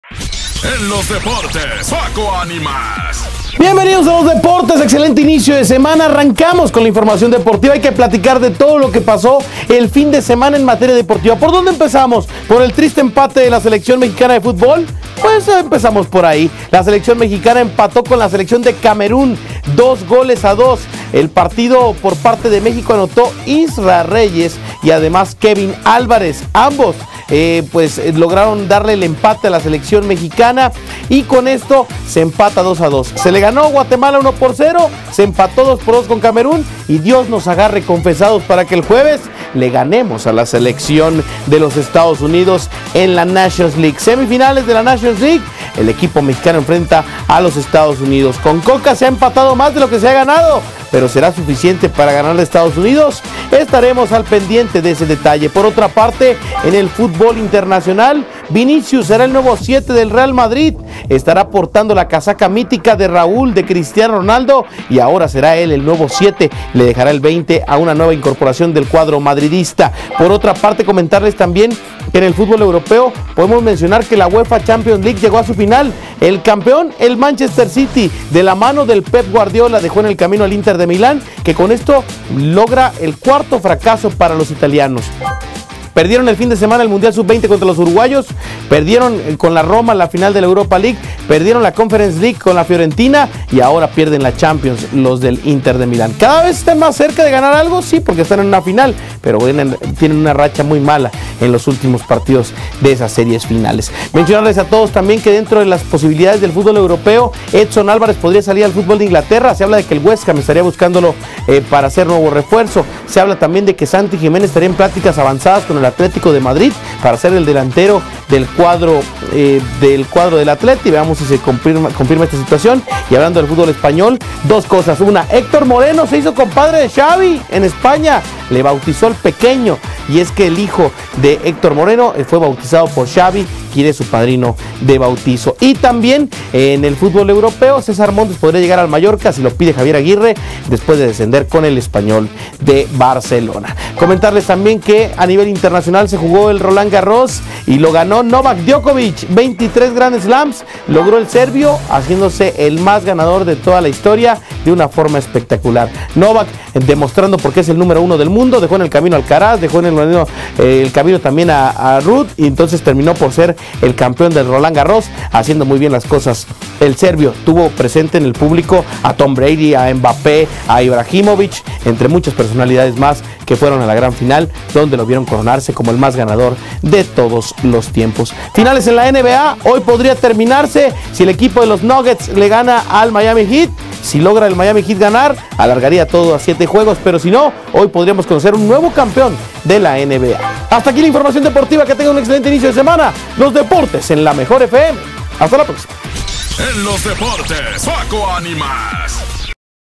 ¡Bienvenido Vinicius! Llega nuevo elemento al Real Madrid y Manchester se corona campeón
Selección Mexicana empata, llega nuevo elemento al Real Madrid, Manchester City campeón y más con Paco Animas en LosDeportes de LaMejor977 😱⚽️